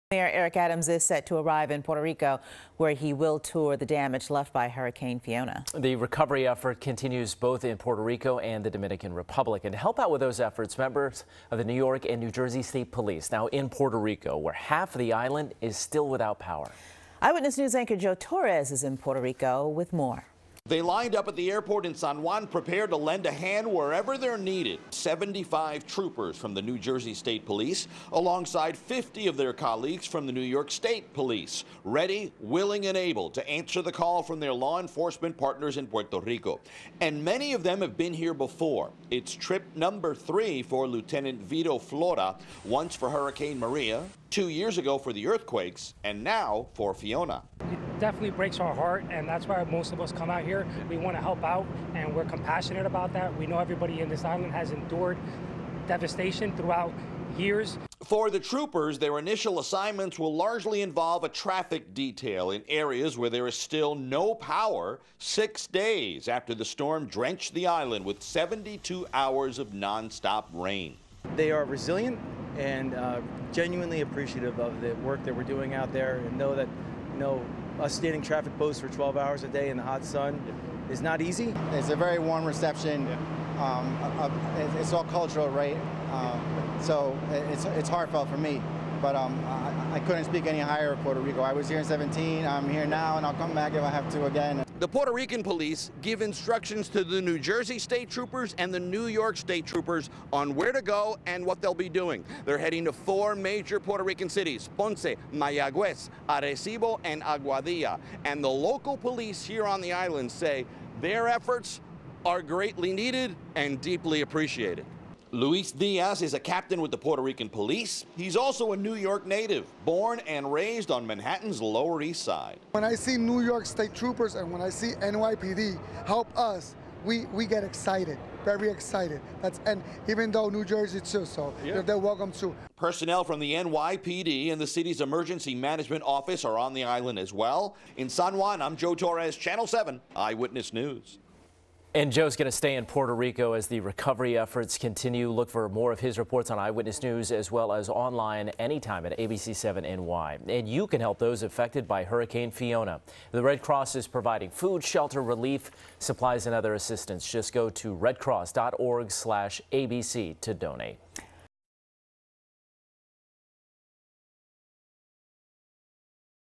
Mayor Adams heads to Puerto Rico to survey damage
Joe Torres has the lastest on Mayor Adams' visit to Puerto Rico as troopers from New York and New Jersey aid in the cleanup effort.